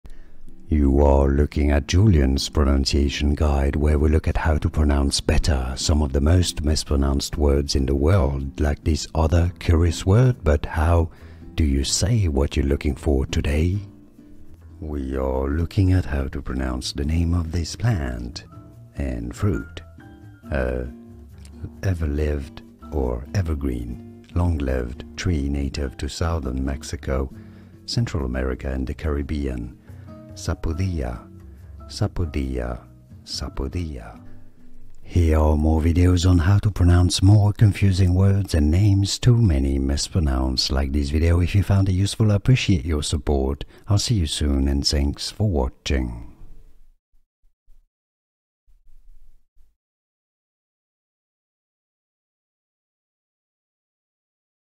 How to Pronounce Sapodilla (Correctly!)
Welcome! Here you will hear How to Pronounce Sapodilla (Correctly!) i.e. the “correct” pronunciation.

You can skip the intro through the time stamps below:
00:00 - Pronunciation Intro
00:15 - How to Pronounce Sapodilla (Correctly!)